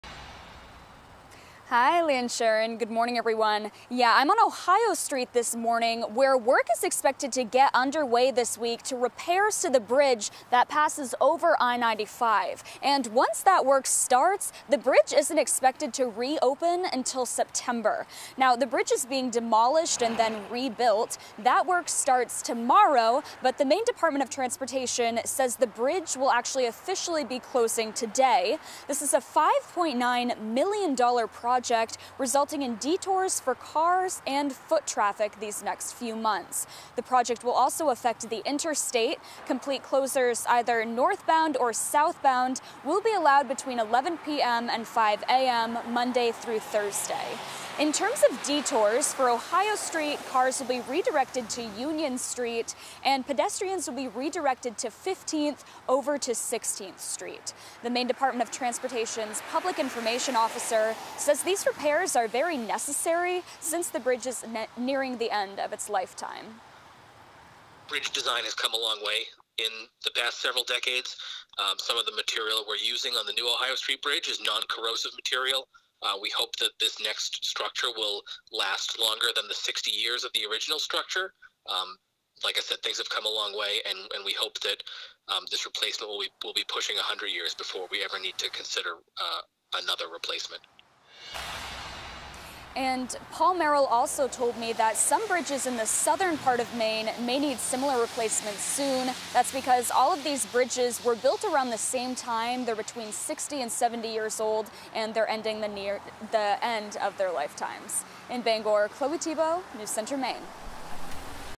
Ohio Street Bridge now closed in Bangor
Work is underway on the Ohio Street Bridge in Bangor. The bridge, which passes over I-95, is being demolished and then rebuilt.